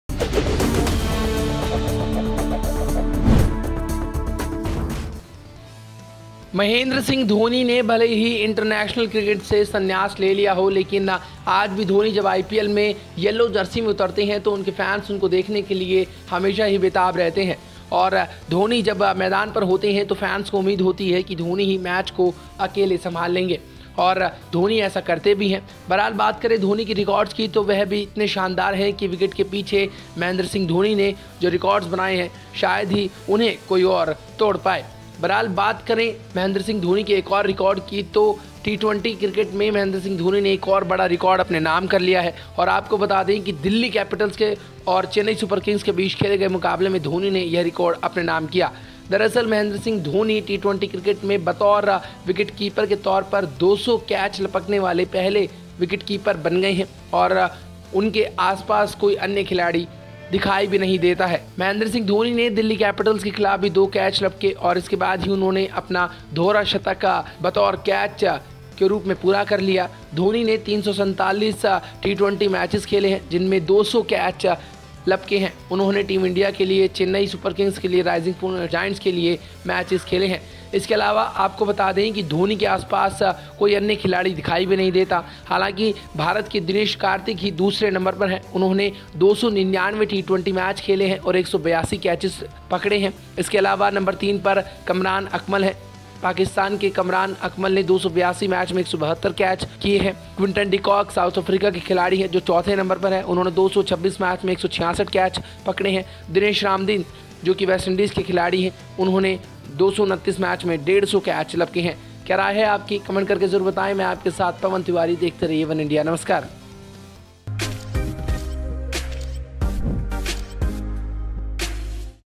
IPL 2022: MS Dhoni के नाम जुड़ा एक और बड़ा रिकॉर्ड, लगा दिया दोहरा शतक | वनइंडिया हिंदी
Former India captain Mahendra Singh Dhoni has made another great record in the cricket world. In this matter, he has even beaten the veterans. There is no one present even around Dhoni. Actually, Dhoni has become the first player to take 200 catches as a wicketkeeper in T20 cricket.

टीम इंडिया के पूर्व कप्तान महेंद्र सिंह धोनी ने क्रिकेट जगत में एक और शानदार रिकॉर्ड अपने नाम कर लिया है. इस मामले में उन्होंने दिग्गजों को भी पछाड़  दिया है. धोनी के आसपास भी कोई मौजूद नहीं है. दरअसल, धोनी टी20 क्रिकेट में बतौर विकेटकीपर 200 कैच लेने वाले पहले खिलाड़ी बन गए हैं.